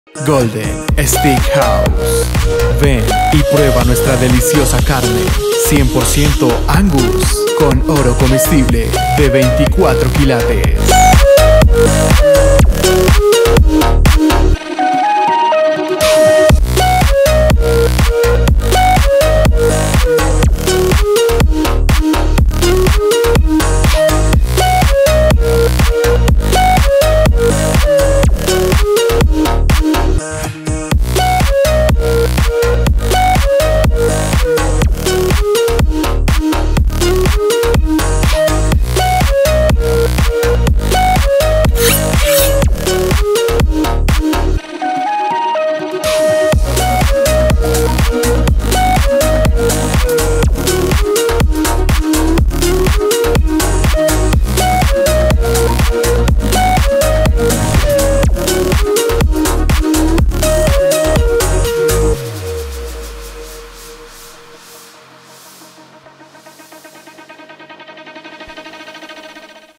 Golden Steak House Popayán - Melodía Films - Sony a6400 + Sigma 16mm - Carne con Oro de 24K
Golden Steak House Popayán

Único sitio en la ciudad de Popayán donde encuentras carne 100% angus con oro comestible de 24K

Dirección: Carrera 6 # 16N - 19 Barrio El Recuerdo
Producción audiovisual: Melodía Films
Dirección: Jhonny Ortiz Fotografía

Melodía Eventos
Tu Aliado en Publicidad y Organización de Eventos